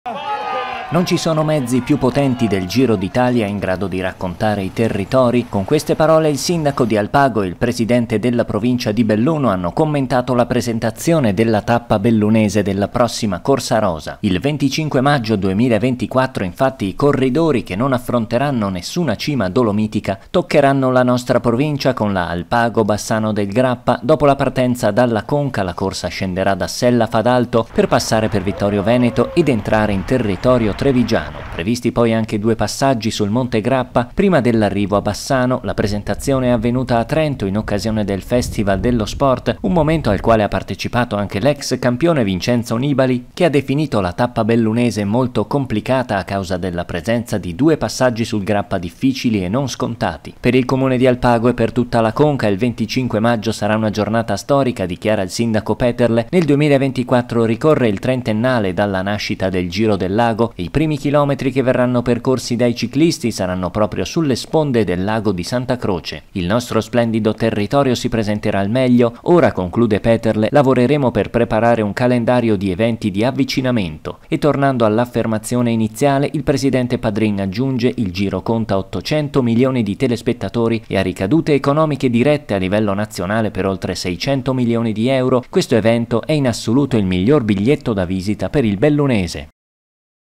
Giro d'Italia: presentata la tappa Alpago - Bassano del Grappa
Presentata la tappa bellunese del Giro d’Italia 2024: ecco la Alpago - Bassano del Grappa.